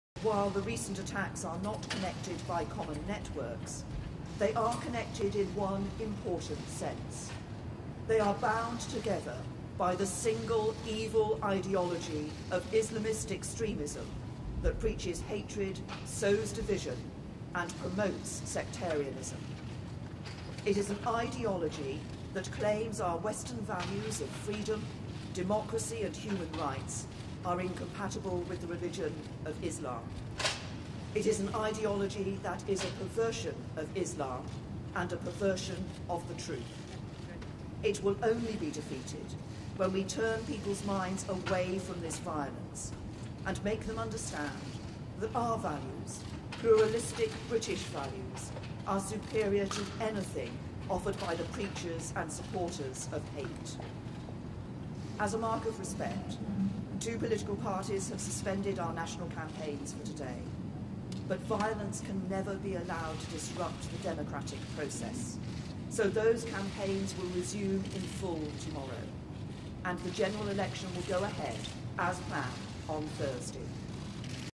Theresa May condemns 'perverted' ideology behind London Bridge terror attack Daily Mail Online
Lastest Showbiz , Sex Naked Very Nice Girl Information World Showbiz